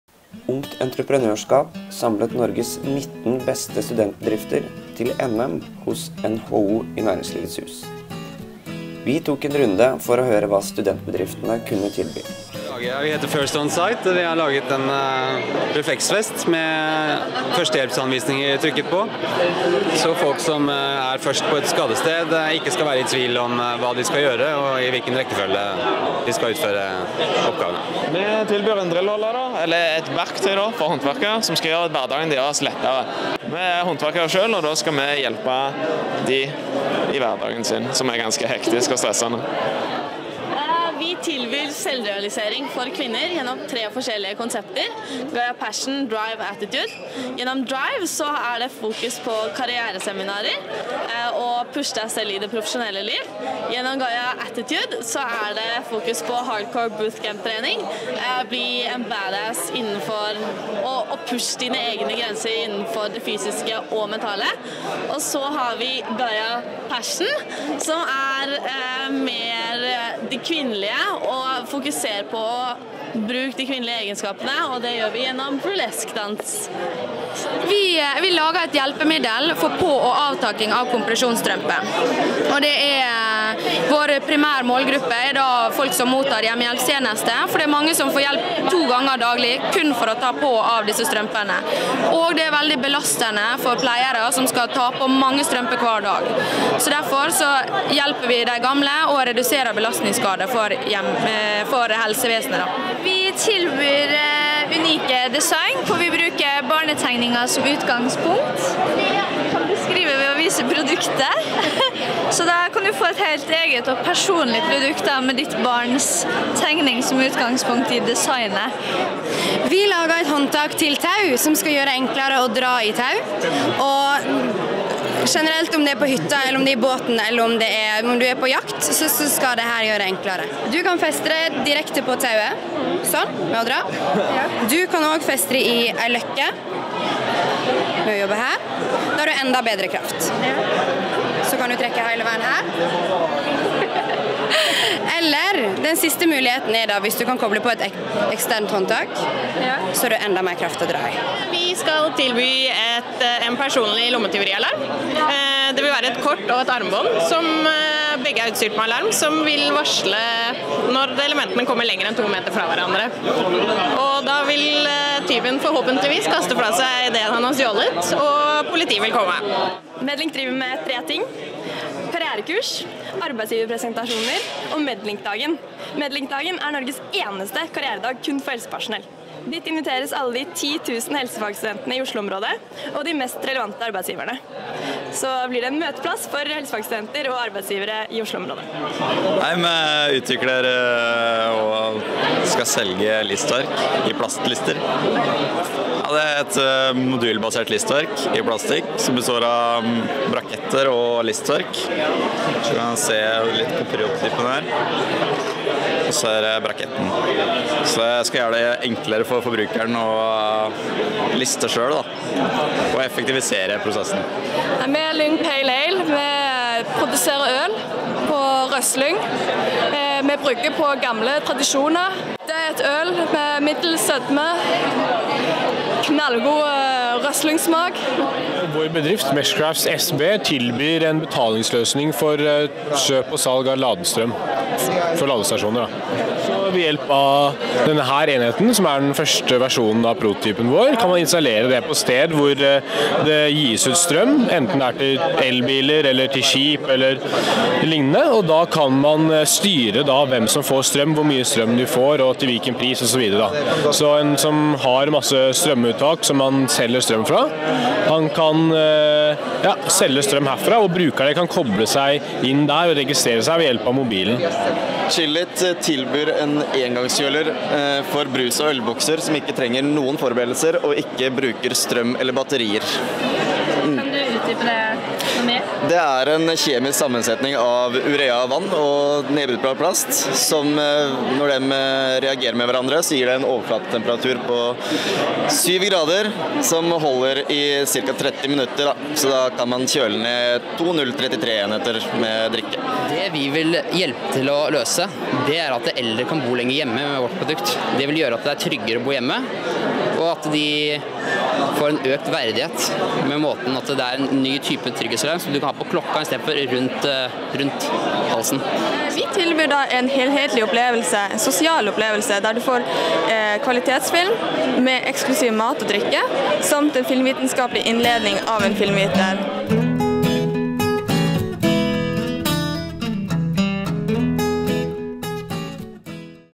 NM for studentbedrifter 2014
Hva tilbyr Norges beste studentbedrifter? NHO TV møtte 14 av dem under NM i regi av Ungt Entreprenørskap Norge. Kanskje kjenner du noen av deltakerne?